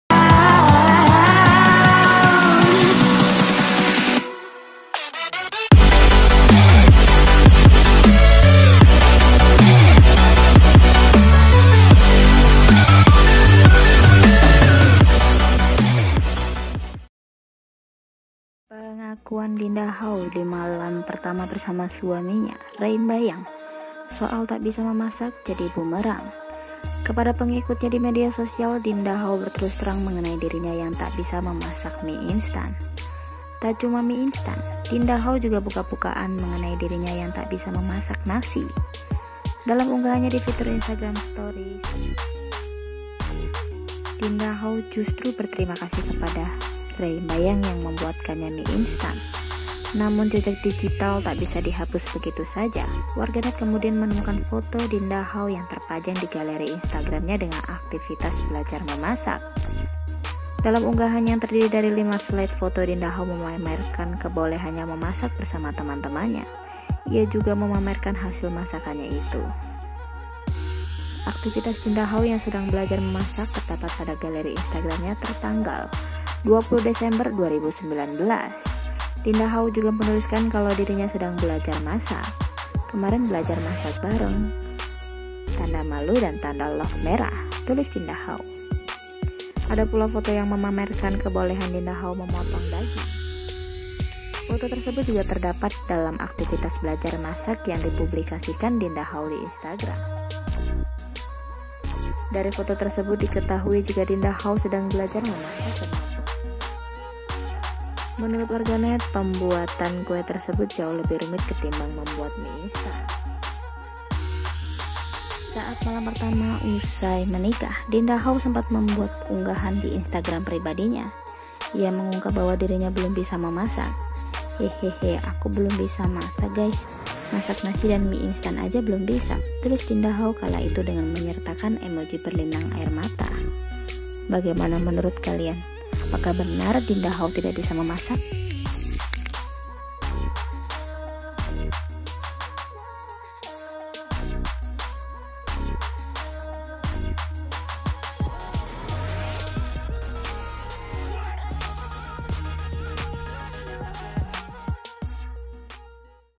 Sebut Dirinya Tak Bisa Masak, Netizen Temukan Bukti Kebohongan Dinda Hauw
Pengakuan Dinda Hauw di malam pertama bersama suaminya, Rey Mbayang, soal tak bisa memasak jadi bumerang.